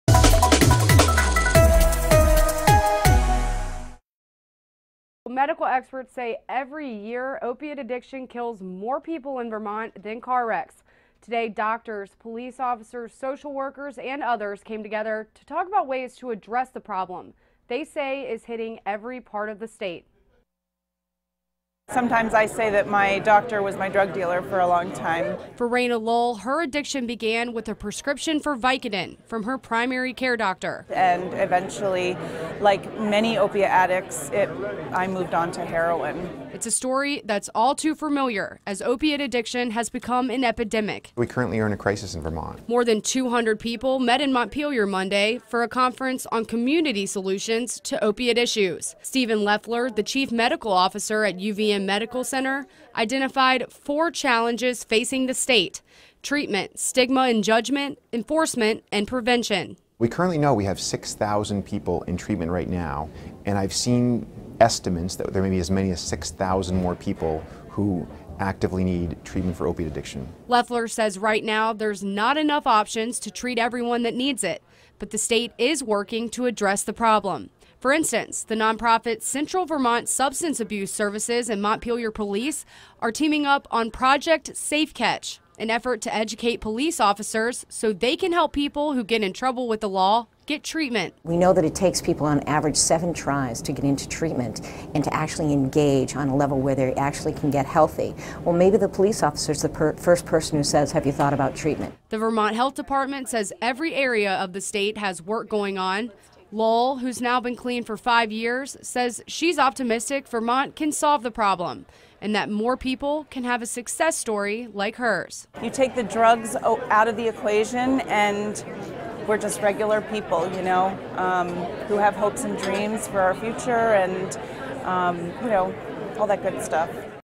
Vermonters meet for conference on state opiate crisis
More than 200 Vermonters discuss challenges and solutions to Vermont's opiate issues.